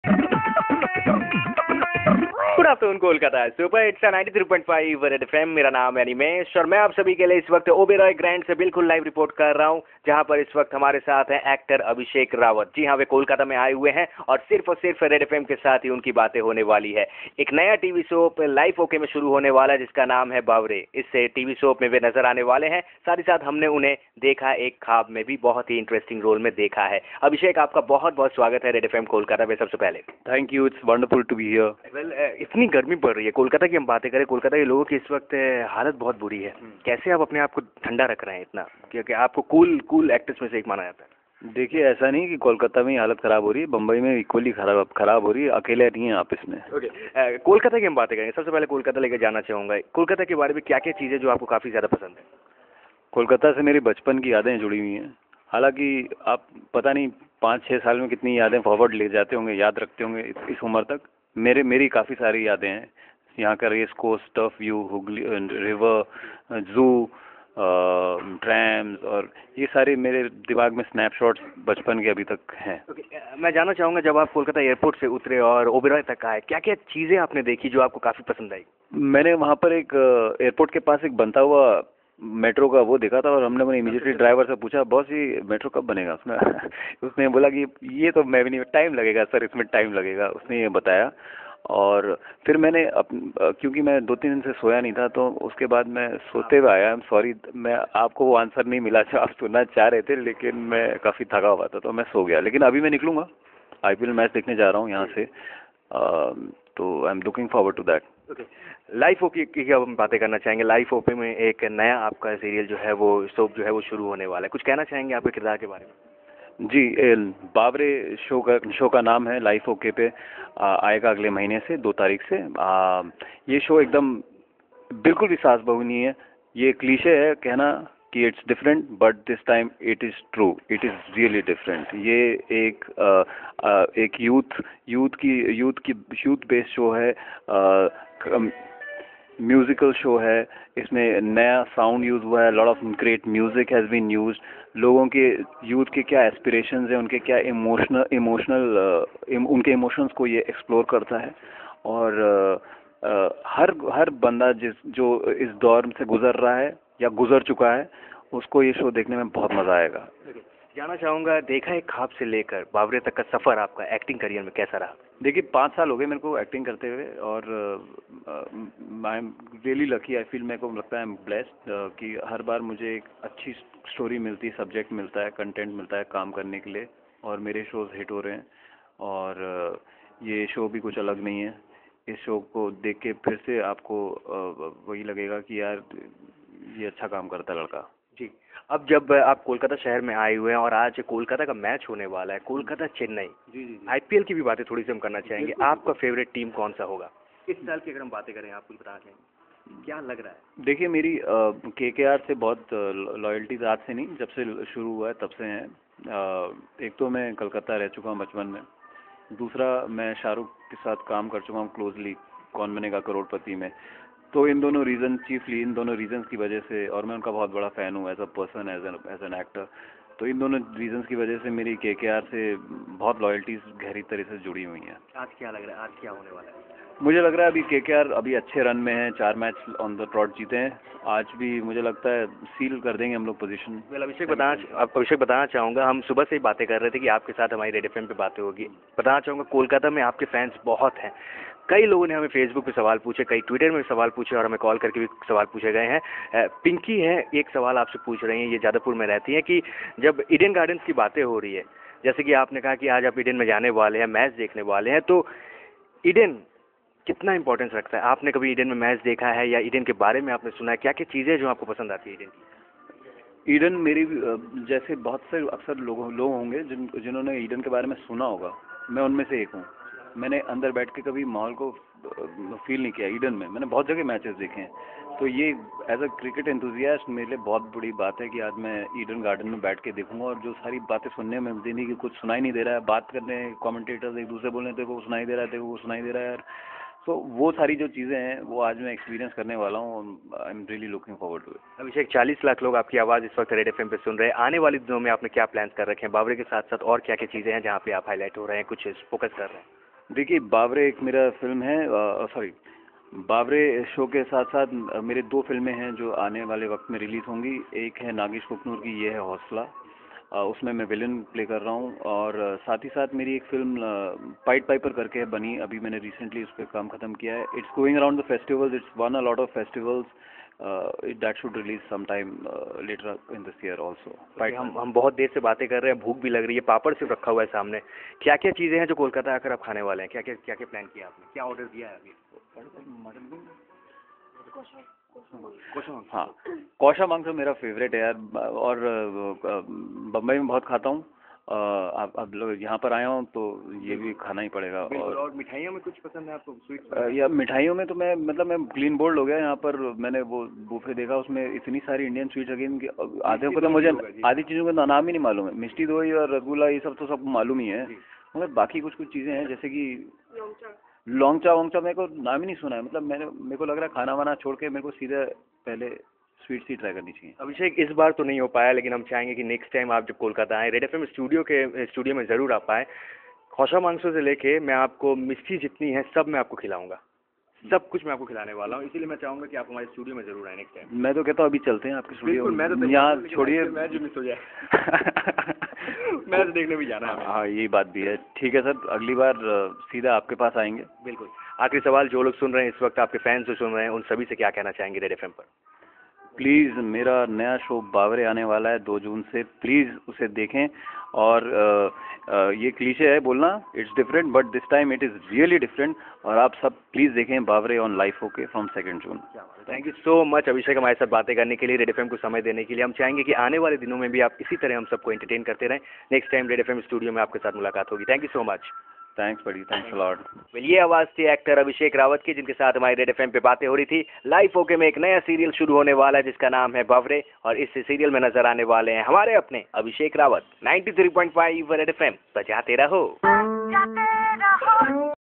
Actor Abhishek Rawat with Rj Animesh on Red fm Kolkata
Promoting his upcoming Tv Soap - Baware on Life OK